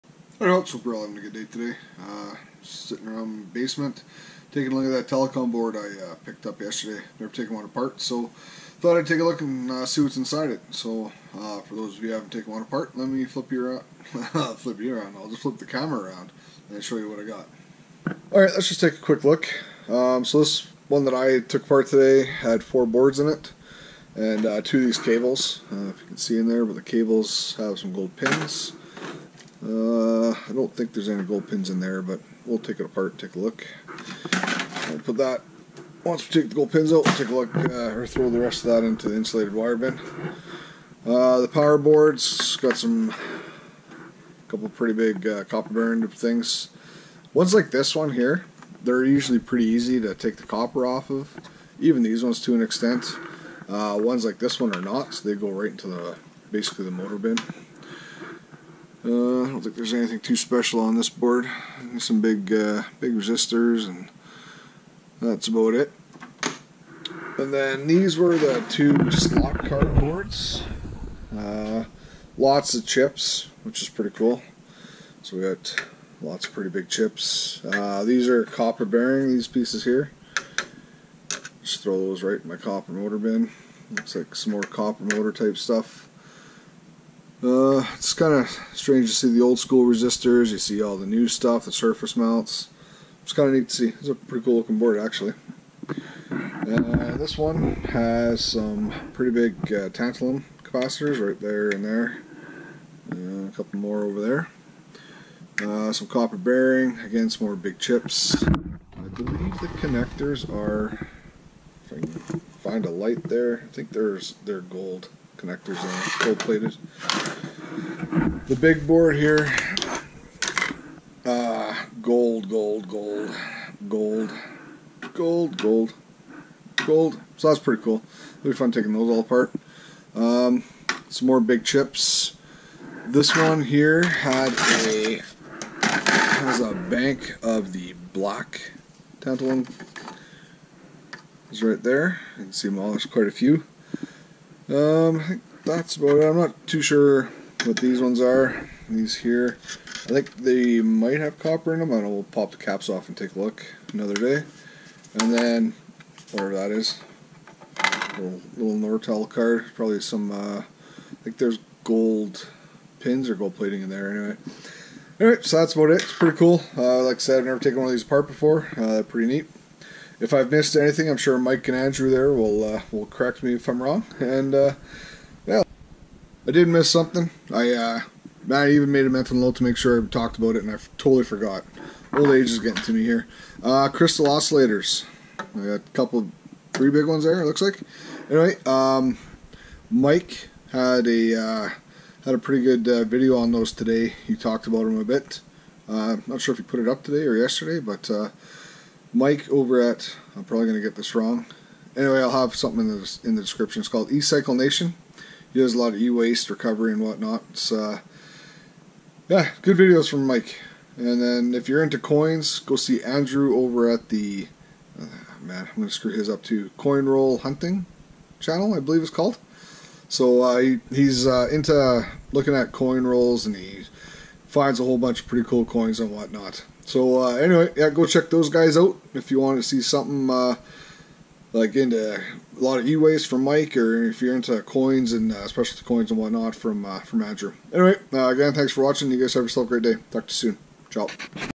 What's In It - Telecom Boards
Howdy Folks,

Took apart that telecom board, pretty cool.

Chris